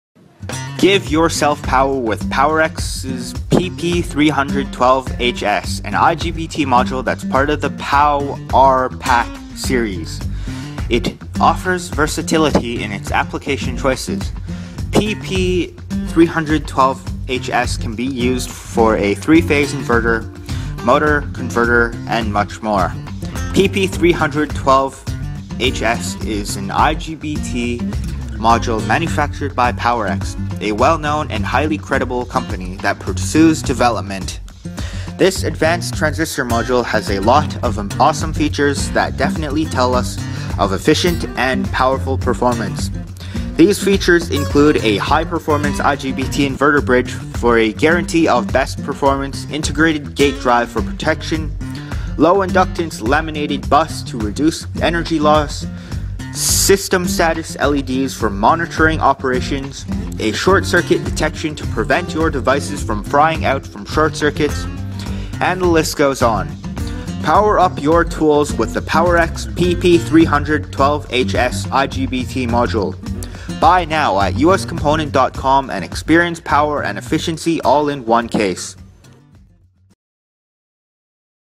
PP30012HS IGBT Power Transistor Module
Power up your tools with the PP30012HS IGBT module! and experience power and efficiency all in one case!

Give yourself power with PP30012HS, an IGBT module that’s part of the POW-R-PAK series. It offers versatility in its application choices -- PP30012HS can be used for a three-phase inverter, motor, converter, and many more.

PP30012HS is an IGBT power transistor module that has a lot of awesome features that definitely tells us of efficient and powerful performance. These features include a high performance IGBT inverter bridge for a guarantee of best performance, integrated gate drive for protection, low inductance laminated bus to reduce energy loss, system status LEDs for monitoring operations, a short circuit detection to prevent your devices frying out from short circuits, and the list goes on!